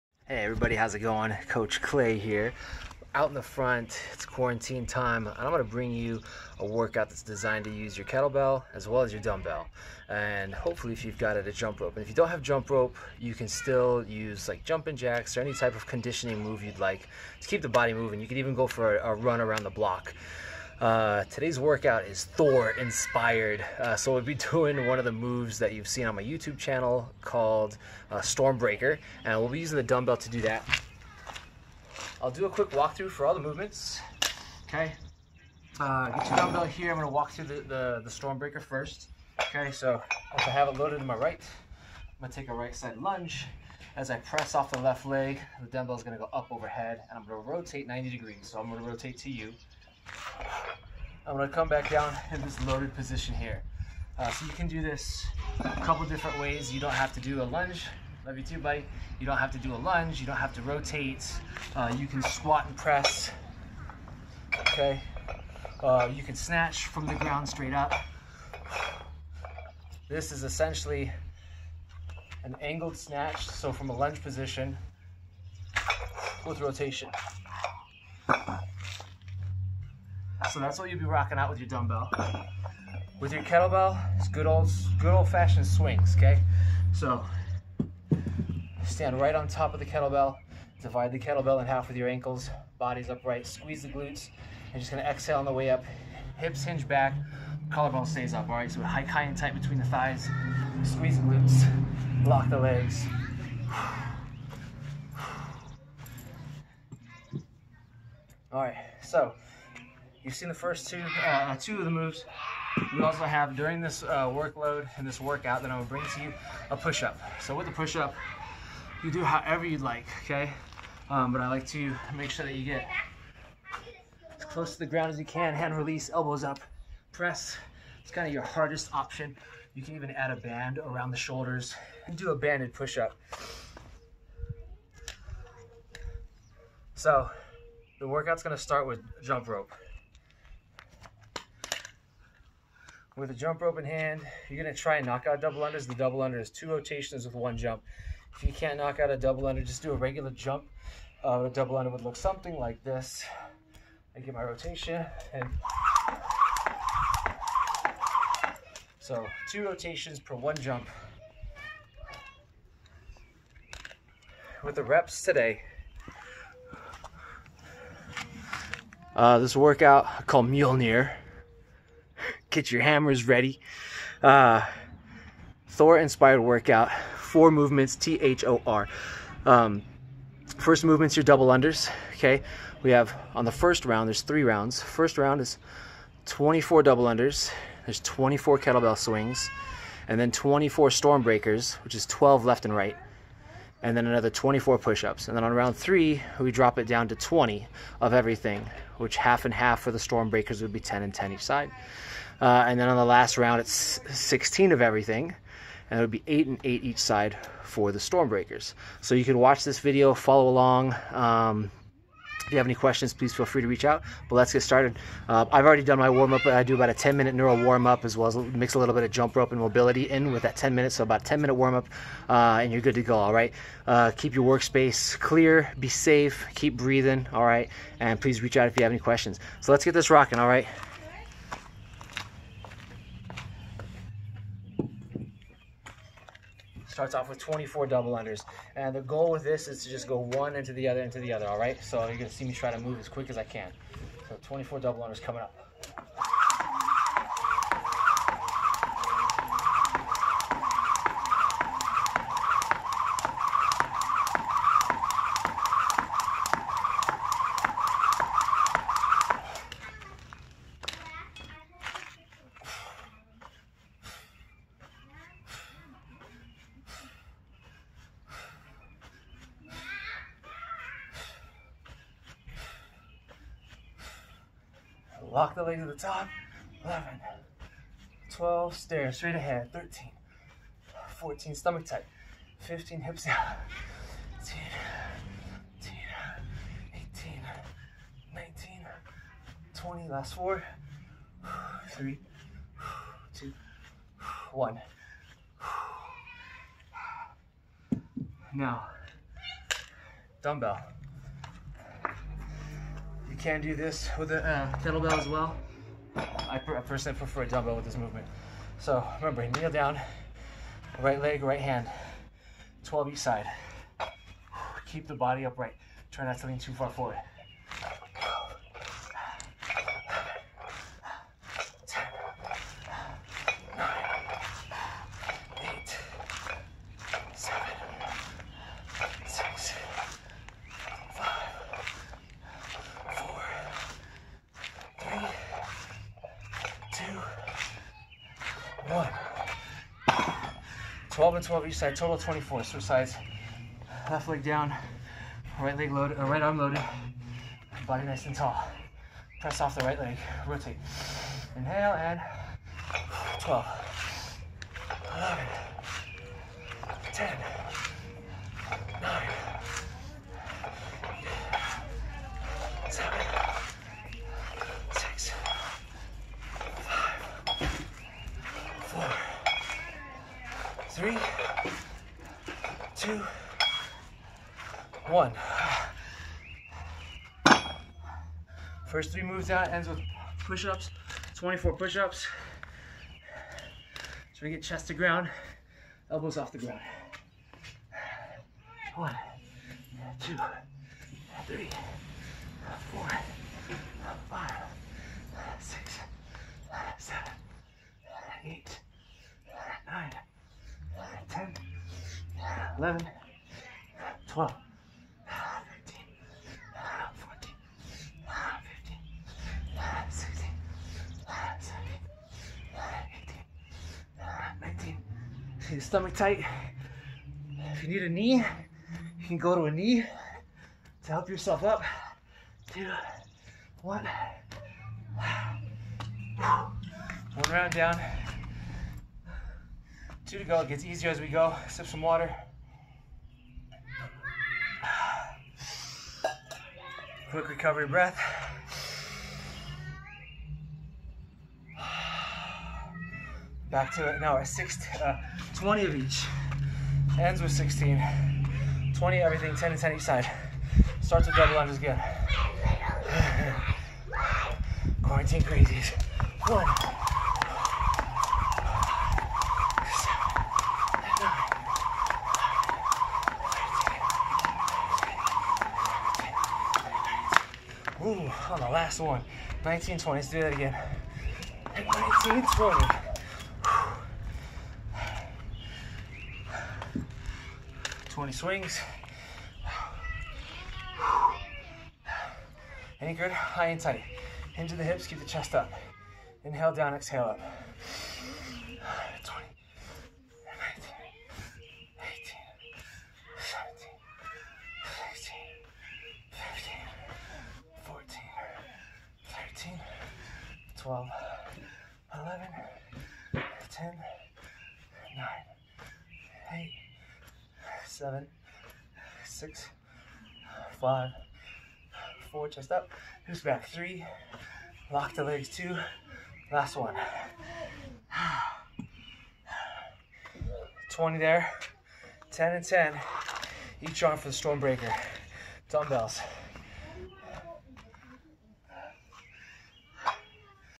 Coach Clay Motley Mjölnir Workout
A Kettlebell, Dumbbell and Jump Rope work best for this training, but are not a necessity. Three round workout. Rest as needed. Repetitions per round are in order as followed - 24/20/16. Movements are: Double Unders - Kettlebell Swings - Stormbreakers (R/L) - Push Ups. Take about 10 minutes prior to ensure you have proper and safe workout area as well as equipment. Warm up for 10 minutes and including mobility. Please consult your primary care physician or doctor before engaging in any questionable physical activity or exercises. Reach out if you have any questions or need alternative recommendations. Thank you very much and Keep Moving Forward! - Coach C.